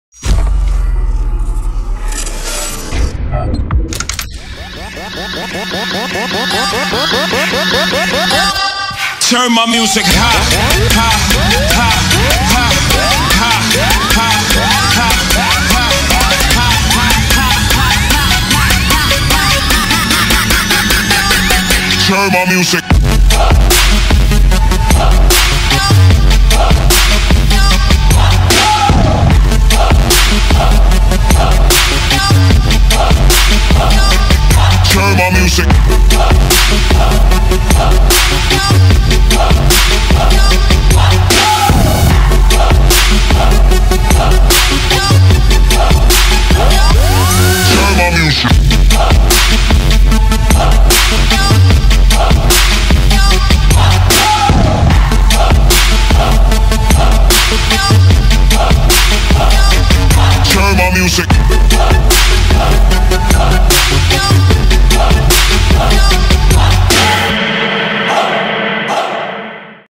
Nissan 370Z on 20" ACE DEVOTION WHEELS / RIMS
Nissan 370Z w/ 20" ACE Devotion Wheels

Vehicle: Nissan 370Z

Wheel Specs: 20x10 Front / 20x10 Rear
Wheels : Ace Devotion - Titanium